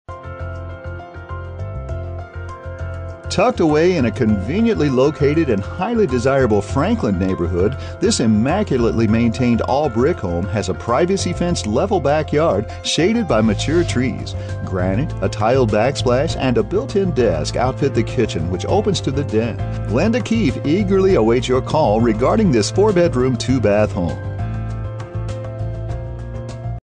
Home for sale in FRANKLIN, TN | $364,900
Don't Miss this Great Home in conveniently located & very desirable Franklin neighborhood! Immaculately maintained all brick home w/privacy fenced level backyard & mature trees.  Large Kitchen w/granite, tile backsplash, built-in desk & open to Den.